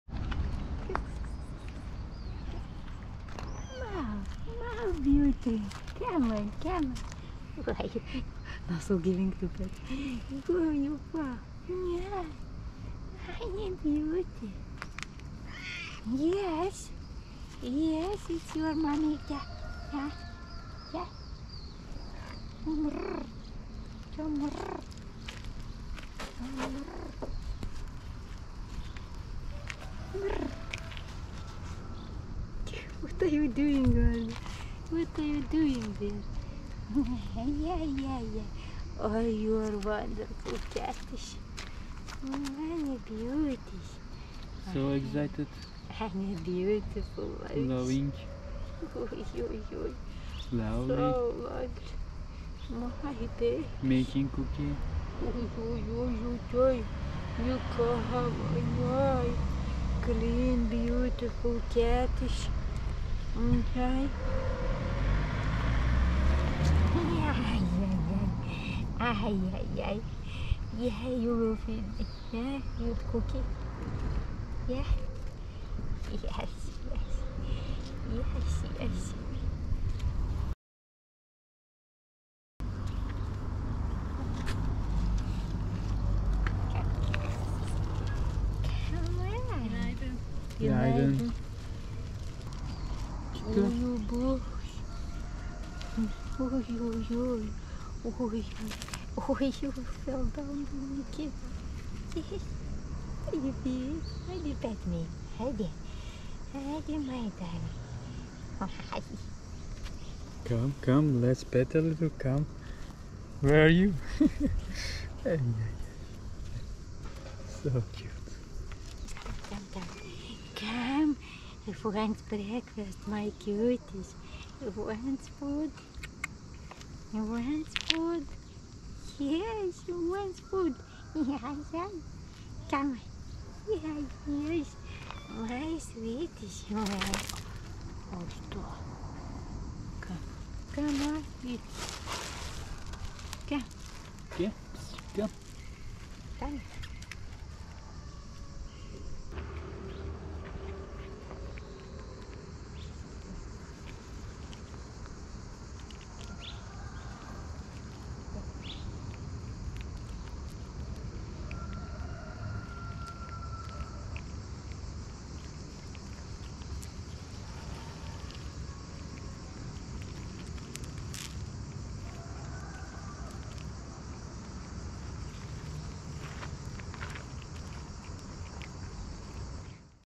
Cute cat was very excited and happy to see me
Cute black and white cat was very excited and happy to see me